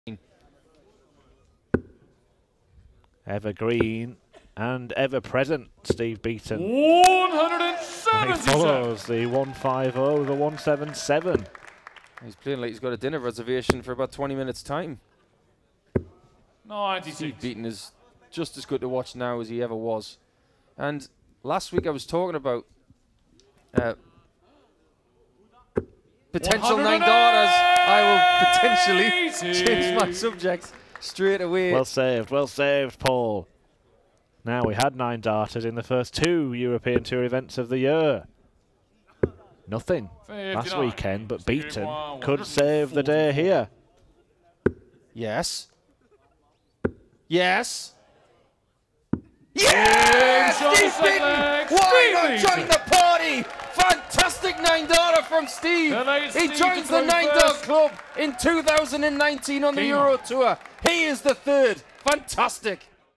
Nine-Darter! Steve Beaton v Kirk Shepherd - 2019 German Darts Open
Steve Beaton fires in a perfect leg in his first round win over Kirk Shepherd at the 2019 German Darts Open.